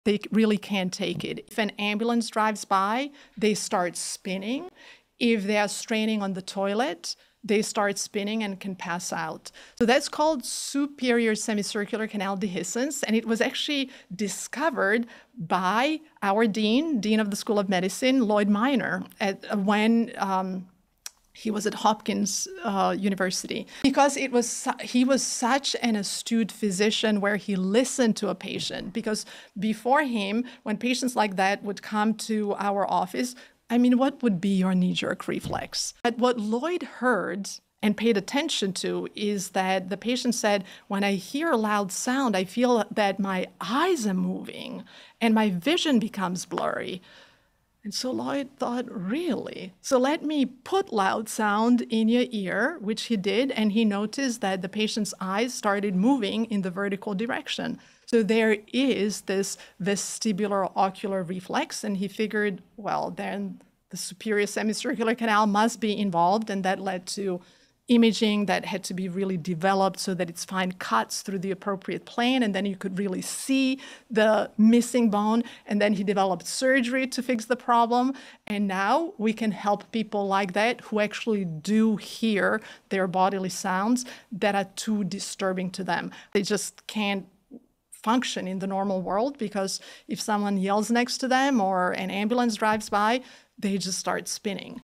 Superior Canal Dehiscence: The Spinning Sensation Explained
An astute physician listened closely to a patient's unusual symptoms: loud noises causing eye movement and blurred vision. This led to the discovery of superior semicircular canal dehiscence and a surgery that restores balance.